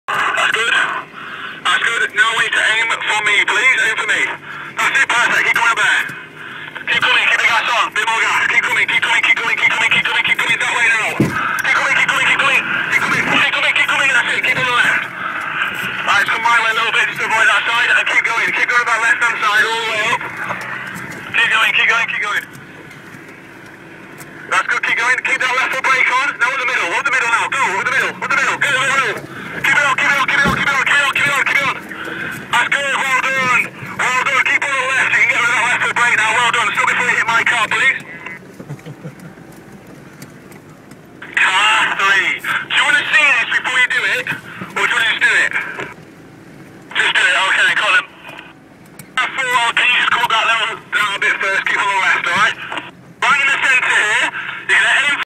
Kankku 4x4 Off-Road Expedition (Part 7)
This is the 7th Part to my Kankku Expedition Video from April 28th, 2012. It is a green-laning 4x4 expedition led by the excellent Kankku team (Dave was our instructor). We travelled in the Lake District lanes, through Grizedale Forest, Coniston area and Langdale. The vehicles in these videos are 2 Kankku branded vehicles - Land Rover Defender and Mitsubishi Pajero. The 2 other cars are own vehicles - Land Rover 90 Defender and a Land Rover Discovery.